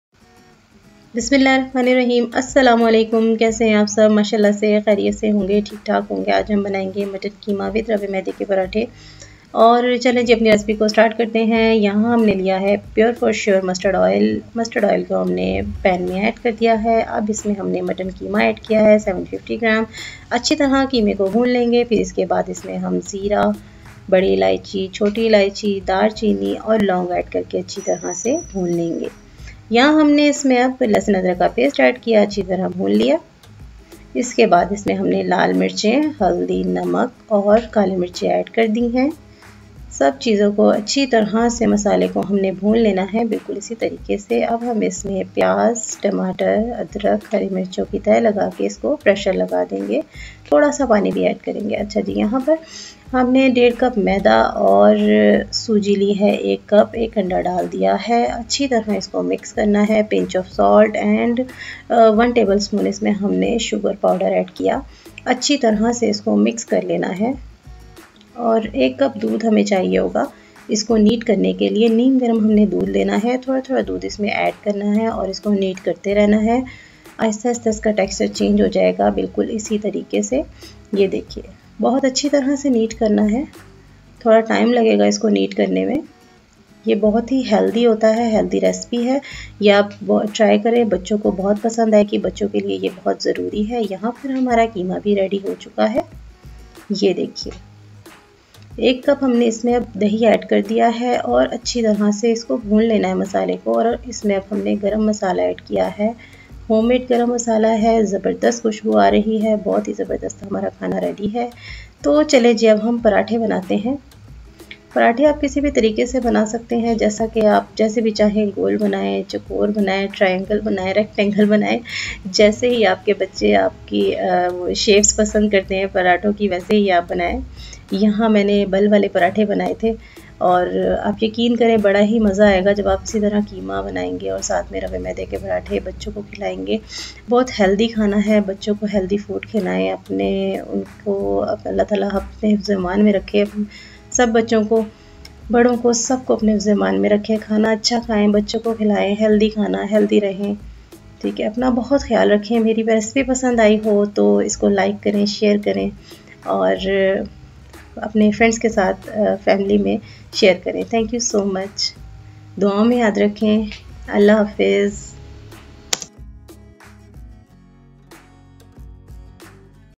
Mutton Qeema with Rava / sooji / semolina maida parathy
today I m making very easy and delicious qeema recipe with
rava maida k parathy
this one of the and healthy recipe friends ♨👍👍
ingredients given below

qeema   750 gm
black cardamom    2 piece
green cardamom    2 piece
cinnamon stick       2 inch
cloves        6 piece
tomatoes     2 big size
ginger and green chili      as your choice

Rava / Soji / Semolina with maida paratha

maida/ all purpose flour/ plain flour       1 1/2 cup
rava / soji / semolina       1 cup